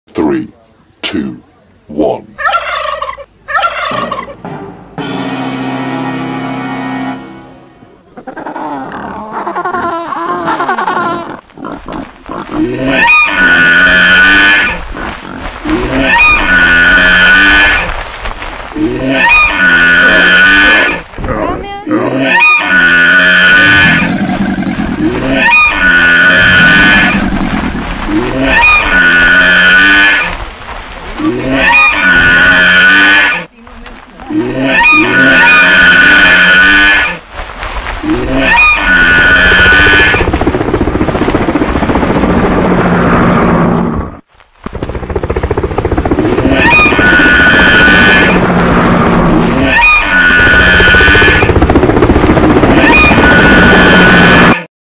learn about cute giraffes#shorts
learn about cute giraffes#shorts The giraffe or zarafah is an even-toed ungulate mammal endemic to Africa and is the tallest animal species living on land. Male giraffes can reach a height of 4.8 to 5.5 meters and can weigh up to 1,360 kilograms. Female giraffes are usually slightly shorter and lighter